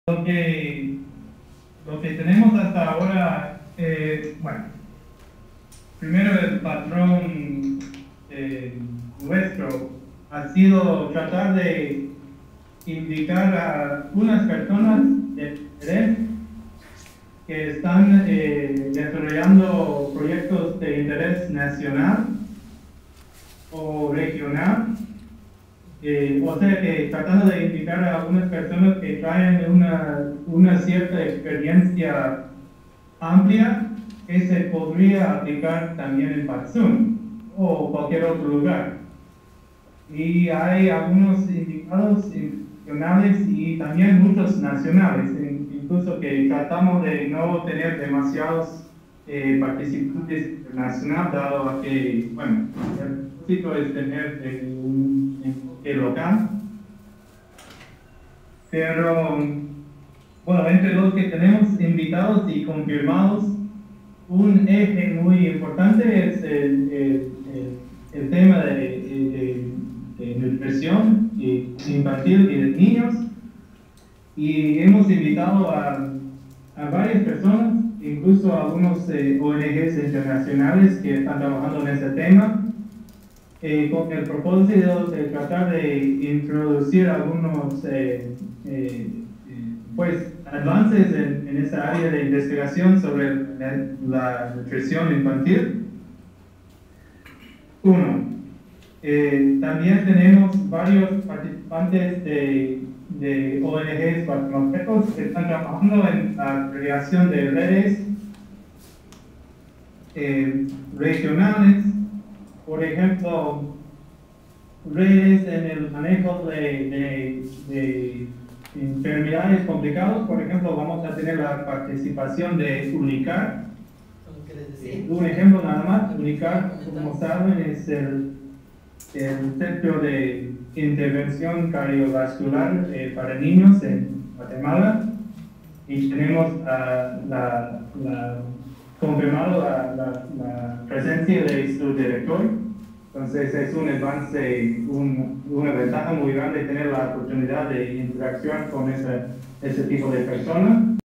Futuros Colectivos, reunión preparatoria Patzún
Patzún, reunión preparatoria de Futuros Colectivos con organizaciones y vecinos de Patzún